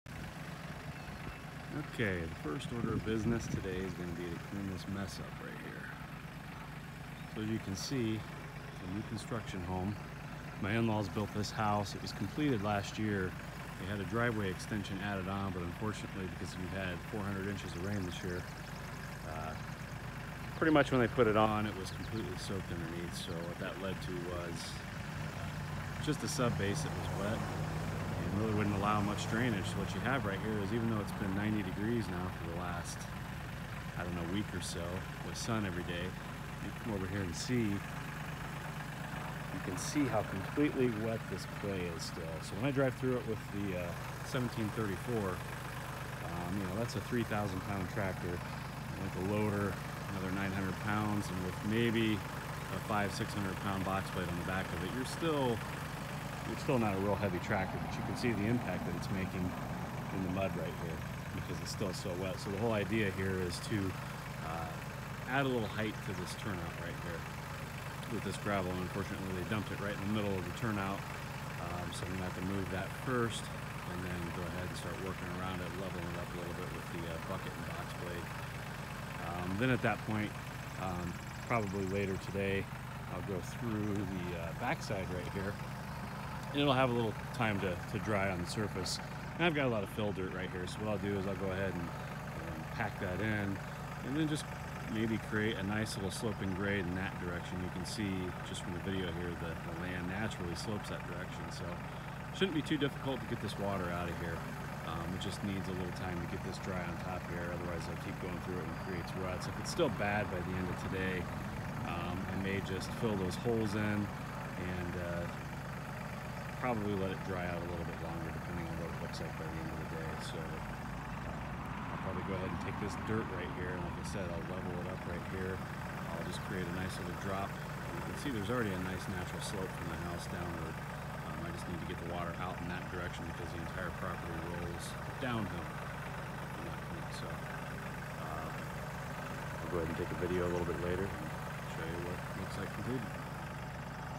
Installing New Driveway Turnout with Compact Tractor
I talk about adding a drive way turnout and the steps I will take to do it.  I am using a Massey 1734 Tractor and a King Kutter Box Blade  The turnout was already cut, and has partial base fill.  As you can see, it was left a mess by the contractor and needs a lot of attention.  This was Spring, 2019.